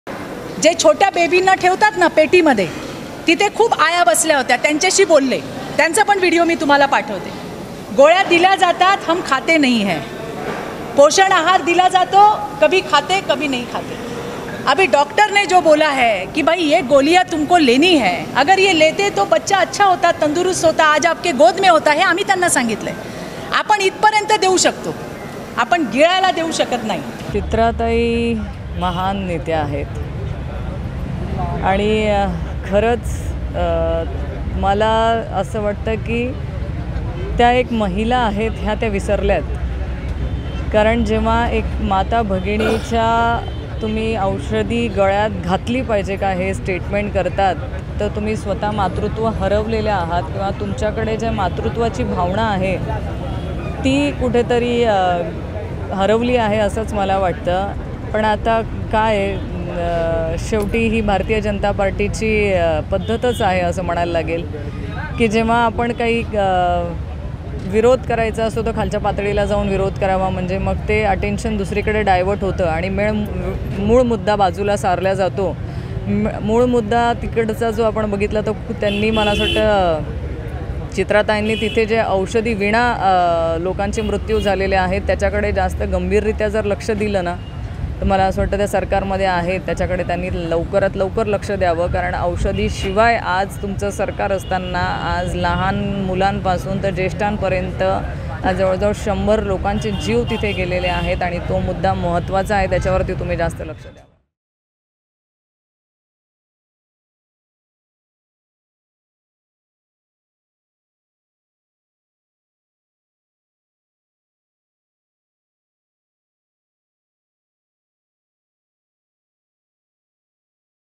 Rohini Khadase on Chitra Wagh : चित्रा वाघ यांच्यातल्या मातृत्वाची भावना हरवली... | LetsUpp Marathi
भाजप नेत्या चित्रा वाघ या महान नेत्या आहेत. त्या एक महिला आहेत, ही गोष्ट त्या विसरल्या आहेत. त्यांच्यातले मातृत्व हरपले आहे का? अशी कडाडून टीका शरद पवार गटाच्या प्रदेशाध्यक्ष रोहिणी खडसे यांनी केली.